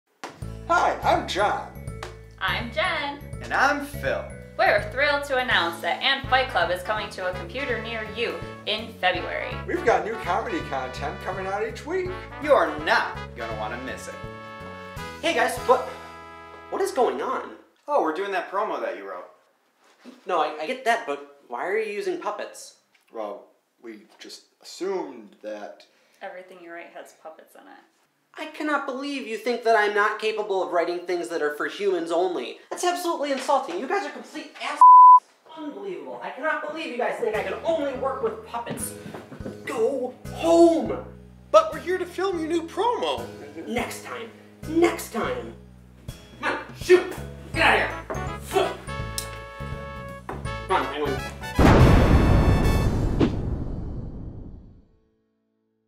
Cam Wrote The Promo - An Fight Club
When we let Cam write a promo, things get furry.

Starring:
Phil Bruno
Cameron Garrity
Jennifer Klemann
JT Szablewski

Adam Kreutinger - Arlo The Monkey
Matthew Krygier - Leslie

Written and Directed by Cameron Garrity
Director of Photography - Van Dinh
Edited by JT Szablewski

@anfightclub on Twitter